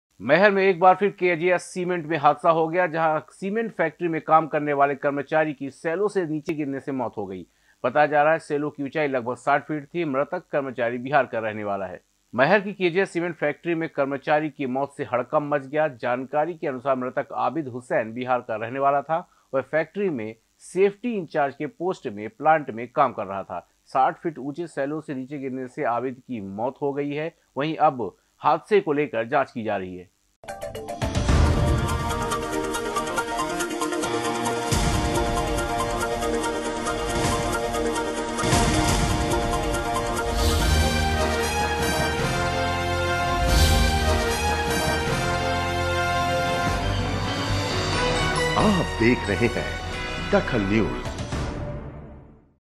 DAKHAL NEWS फिर विवादों में आई KJS सीमेंट कंपनी
फिर विवादों में आई KJS सीमेंट कंपनी
60 फीट ऊँचे सैलो से गिरा कर्मचारी